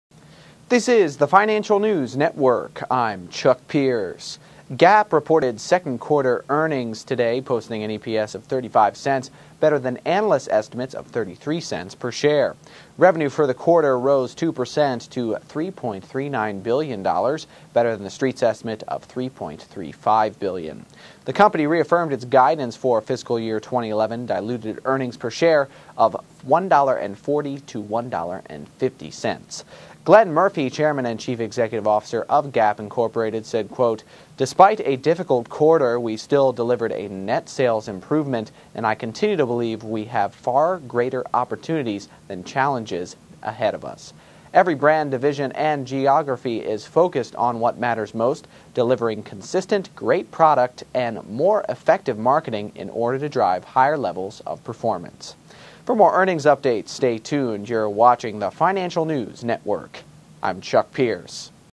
Gap Topped Estimates For Q2, Rose 2% YoY
Gap (NYSE:GPS) reported Q2 EPS of $0.35, better than analyst estimates of $0.33 per share. Revenues for the quarter rose 2% to $3.39 billion, better than consensus estimates of $3.35 billion.

The company reaffirmed its guidance for fiscal year 2011 diluted earnings per share of $1.40 to $1.50.

Glenn Murphy, chairman and chief executive officer of Gap Inc. said, "Despite a difficult quarter, we still delivered a net sales improvement and I continue to believe we have far greater opportunities than challenges ahead of us.  Every brand, division, and geography is focused on what matters most - delivering consistent, great product and more effective marketing in order to drive higher levels of performance."